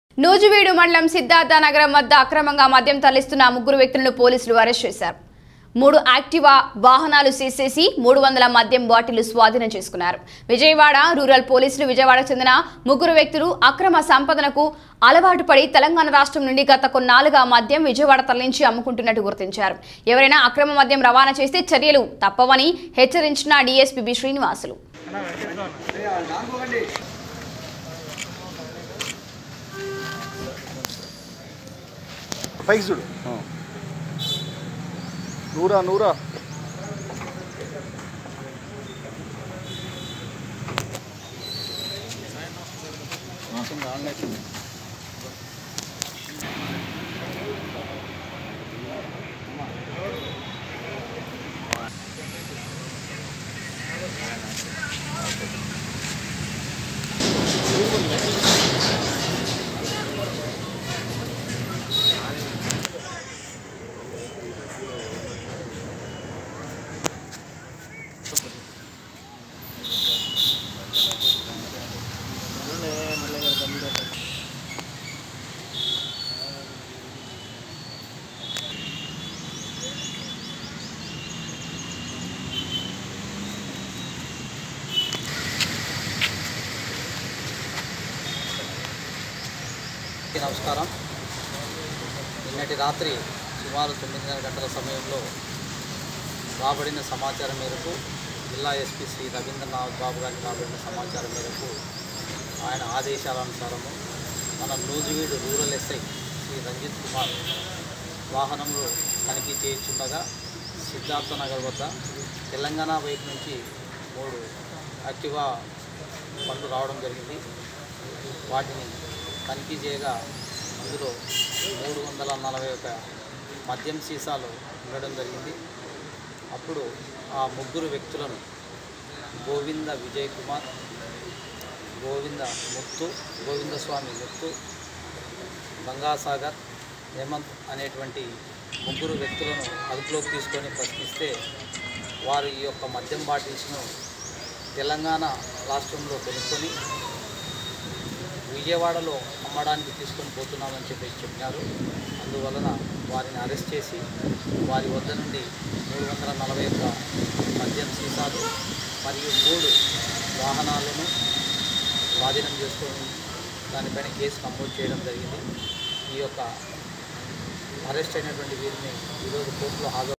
నూజివీడులో అక్రమంగా మద్యం తరలిస్తున్న ముగ్గురు అరెస్టు వాహనాలు సీజ్ ||VS9NEWS
Watch:
నూజివీడులో.
అక్రమంగా మద్యం తరలిస్తున్న ముగ్గురు అరెస్టు

నూజివీడు మండలం సిద్ధార్థ నగరం వద్ద అక్రమంగా మద్యం తరలిస్తున్న ముగ్గురు వ్యక్తులను పోలీసులు అరెస్ట్ చేశారు. మూడు యాక్టివా వాహనాలు సీజ్ చేసి 300 మధ్యం బాటిళ్లు స్వాధీనం చేసుకున్నారు విజయవాడ రూరల్ పోలీసులు. విజయవాడకు చెందిన ముగ్గురు వ్యక్తులు అక్రమ సంపాదనకు అలవాటు పడి తెలంగాణ రాష్ట్రం నుండి గత కొన్నాళ్లుగా మద్యం విజయవాడ తరలించి అమ్ముకుంటున్నటు గుర్తించారు. ఎవరైనా అక్రమ మద్యం రవాణా చేస్తే చర్యలు తప్పవని హెచ్చరించిన డిఎస్పి బి.శ్రీనివాసులు